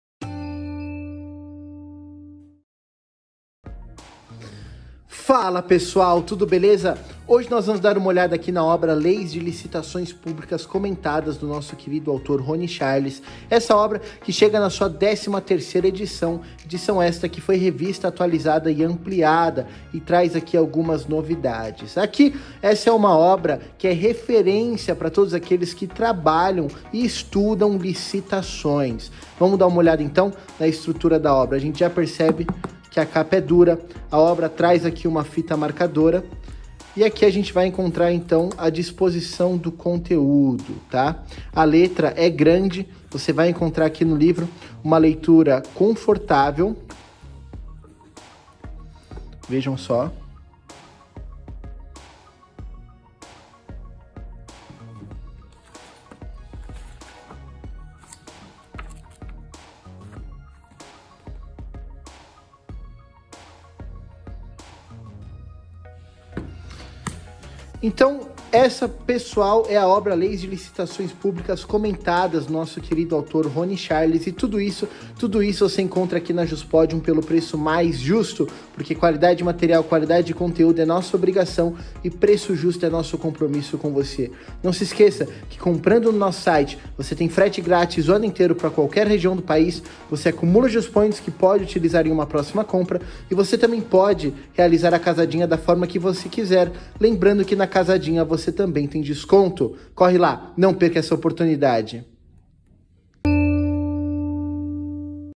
Leis de Licitações Públicas Comentadas (2022)
POR QUE ESCOLHER O LIVRO LEIS DE LICITAÇÕES COMENTADAS?

Em 2021, justamente no dia 01 de abril, foi aprovada a Lei nº 14.133/2021, a Nova Lei de Licitações.
Buscando tratar sobre cada um dos dispositivos desta Nova Lei, além dos comentários aos dispositivos licitatórios da LC 123, de 2006, produzimos intensa pesquisa, realizando comentários, artigo a artigo, com reflexões e propostas para a compreensão da norma jurídica que será construída a partir do texto aprovado pelo legislador.
O esgotamento da décima segunda edição, e das tiragens posteriormente publicadas, estimulou a confecção desta nova edição, que abordará as primeiras Jurisprudências sobre a aplicação da Lei nº 14.133/2021, posições doutrinárias e normatizações recentemente publicadas, trazendo novos elementos para o estudo do tema.
Mantivemos o novo modelo de formulação dos comentários, facilitando a pesquisa dos leitores. Assim, os comentários artigo a artigo adotam, em relação à Lei nº 14.133/2021, numeração de tópicos baseada no artigo comentado, com desenvolvimento dos respectivos subtópicos.
Tal modelagem facilitará a pesquisa e permitirá uma consulta ágil e direta, a partir da leitura do artigo e seus respectivos comentários, assim como também a leitura a partir do sumário, que permite uma visão geral dos temas abordados no livro. A décima terceira edição avança em relação às que a precederam. O livro ficou mais claro, mais didático e mais preciso, em nossa opinião.

O Autor